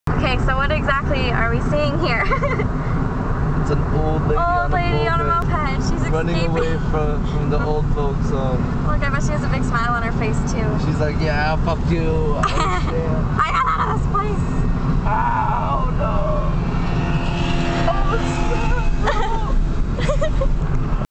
Old Lady Escapes!
Driving to Dance FX, Sara and I saw an old lady on a mo-ped.

It was nuts!